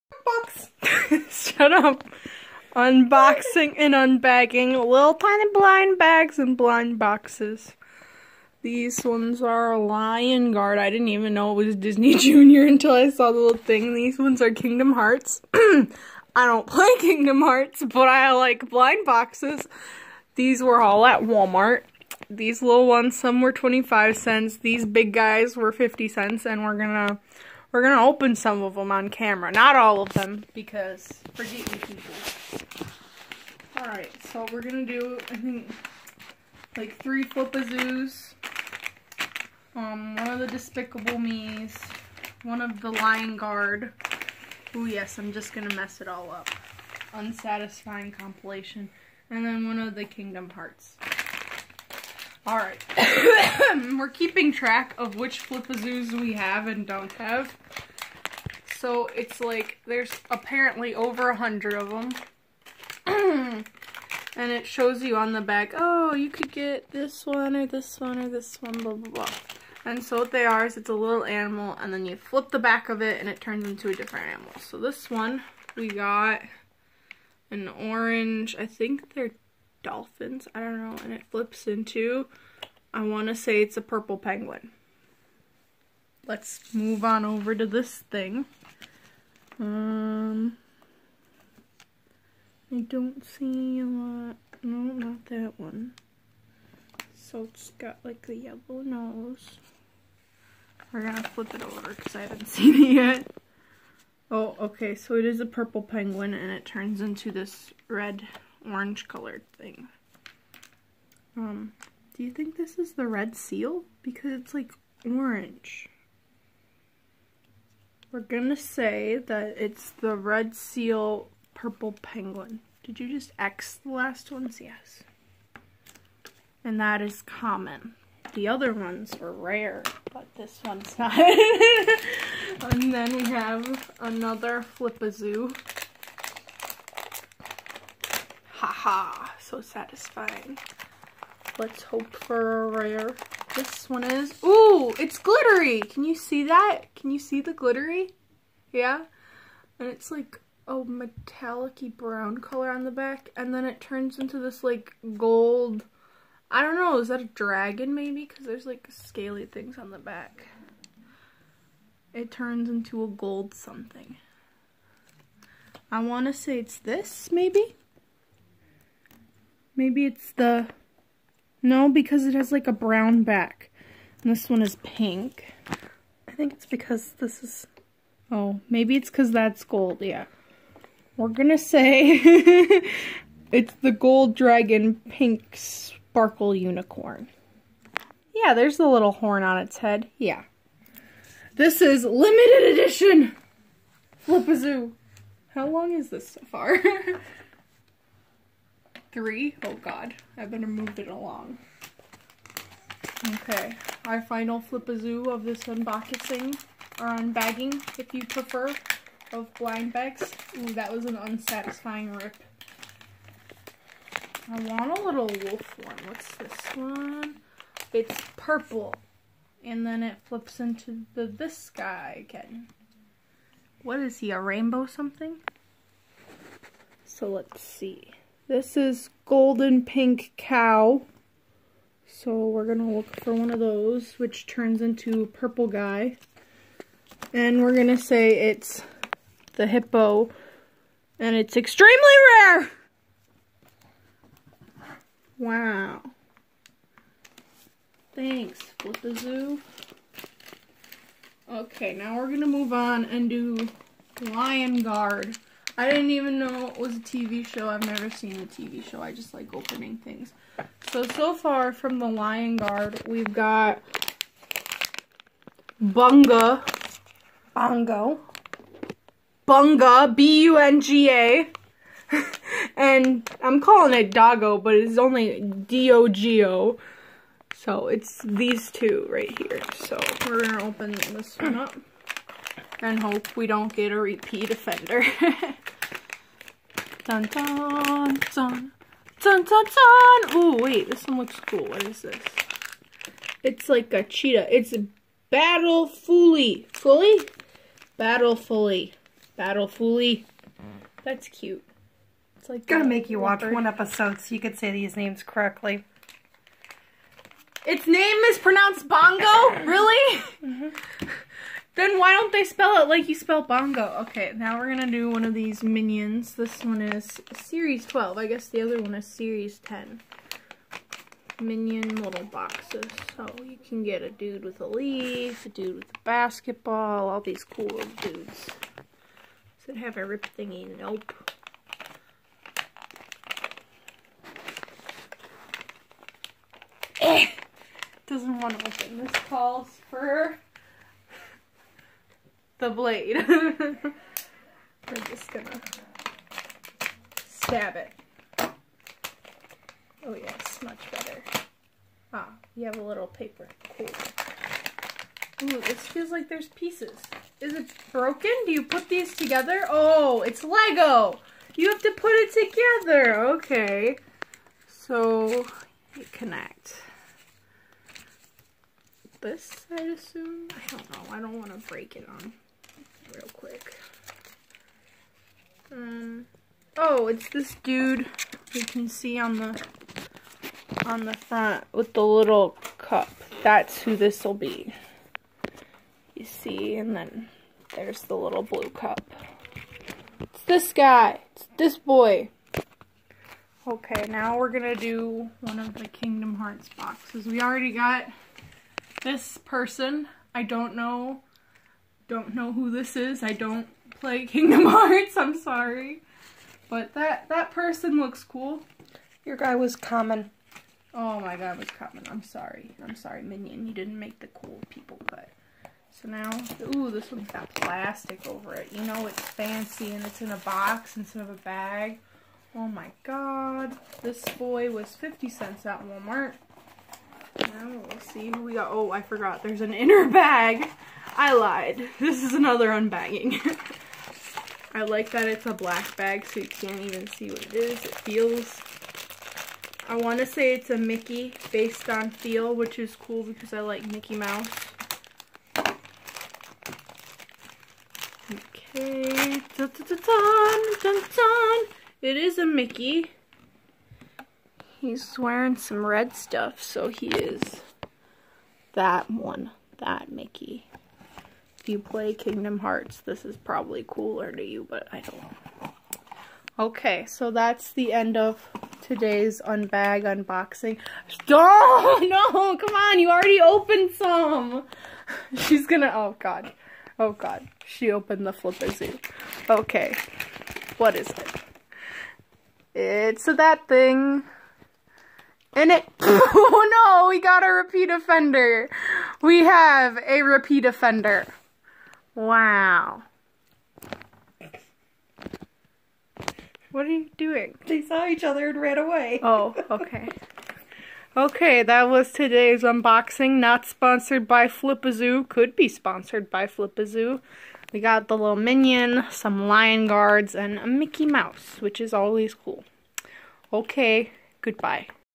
Blind Bag Opening
I opened blind bags a one blind box, theyre Flipazoos, Kingdom Hearts, Lion Guard and Minions